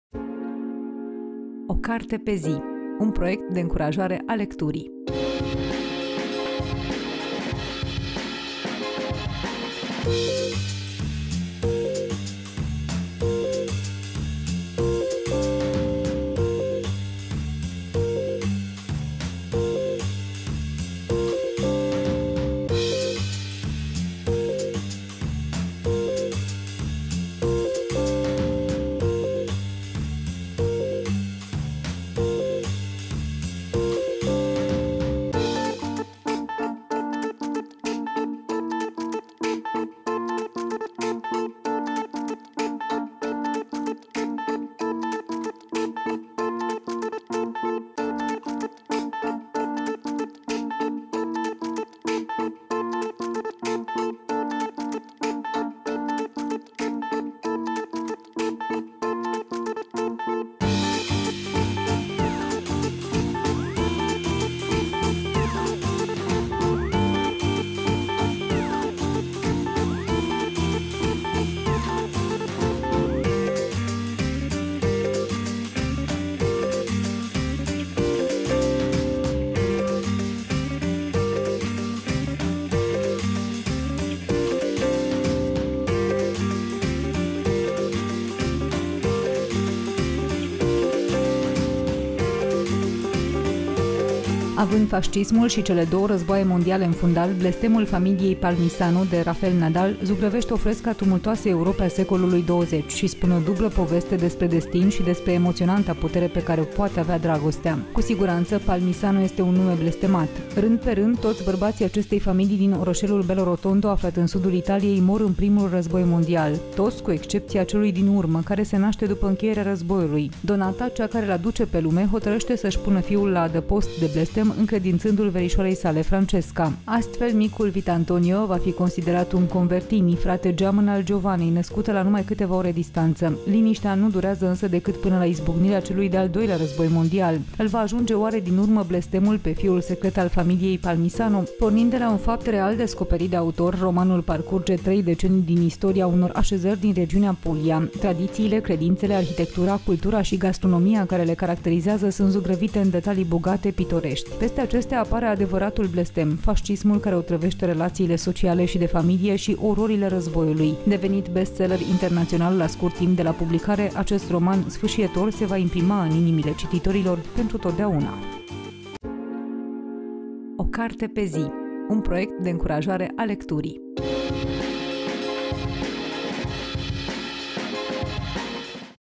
O carte pe zi: „Secretul familiei Palmisano” de Rafel Nadal
„Secretul familiei Palmisano” este recomandarea zilei de 10 mai 2021, în cadrul rubricii „O carte pe zi”.
Cu siguranţă, Palmisano este un nume blestemat: rând pe rând, toţi bărbaţii acestei familii din orăşelul Bellorotondo, aflat în sudul Italiei, mor în Primul Război Mondial. Toţi, până la cel din urmă, care se naşte după încheierea războiului. Donata, cea care îl aduce pe lume, hotărăşte să-şi pună fiul la adăpost de blestem încredinţându-l verişoarei sale, Francesca. Astfel, micul Vitantonio va fi considerat un Convertini, frate geamăn al Giovannei, născută la numai câteva ore distanţă. Liniştea nu durează însă decât până la izbucnirea celui de-al Doilea Război Mondial. Îl va ajunge, oare, din urmă blestemul pe fiul secret al familiei Palmisano?

© un proiect Litera în parteneriat cu Digi24.